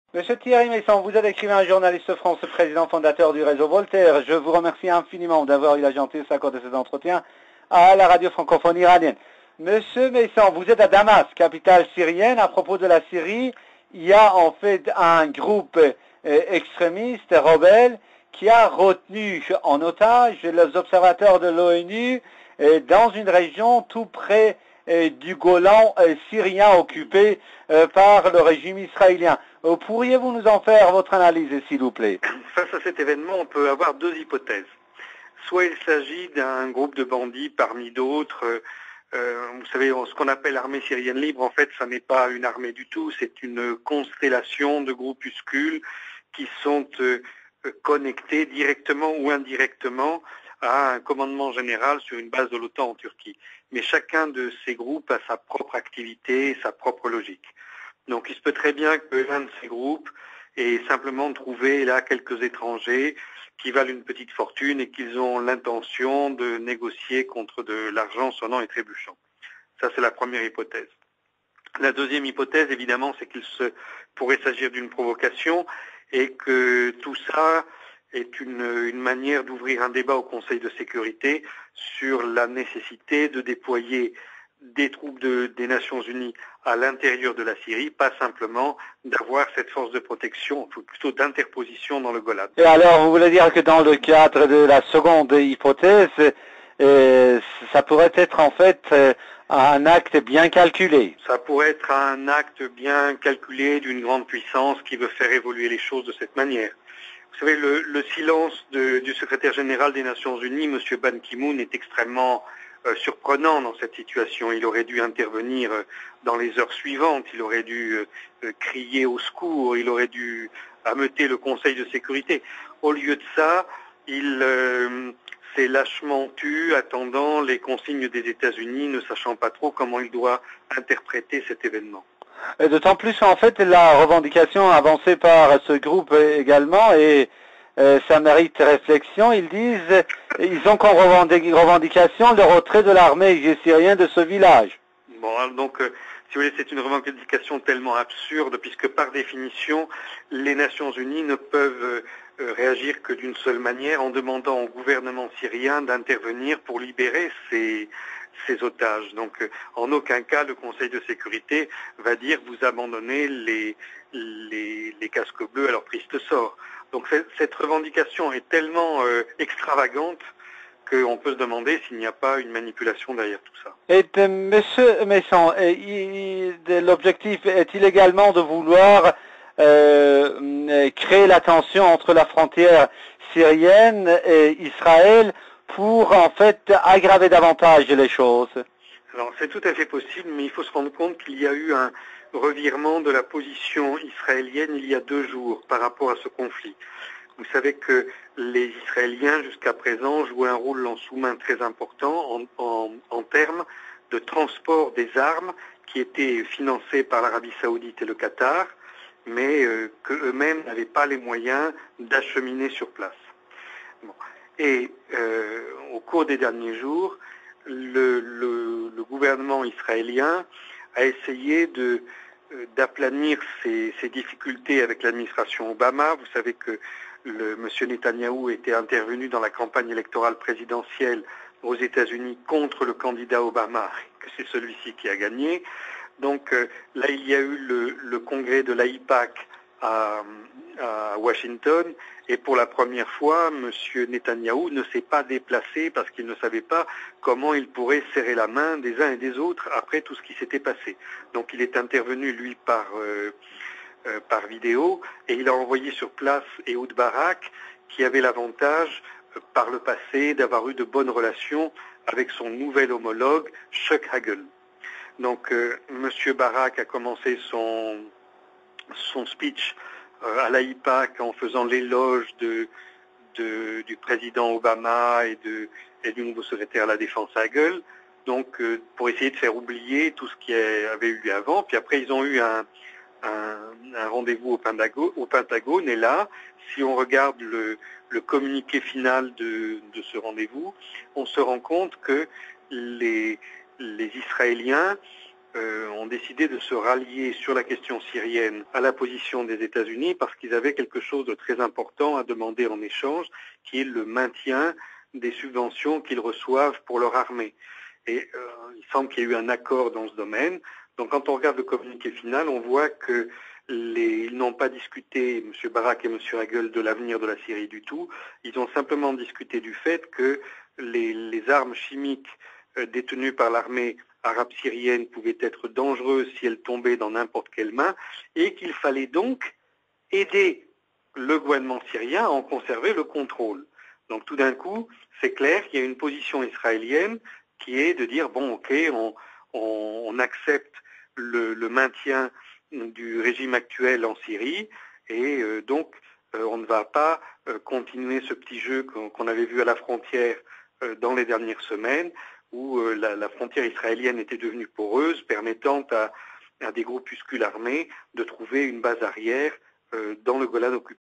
Entretien avec M.Thierry Meyssan "Les observateurs de l'ONU, en otages".
IRIB- Entretien avec M.Thierry Meyssan, journaliste français: "Les observateurs de l'ONU, en otages".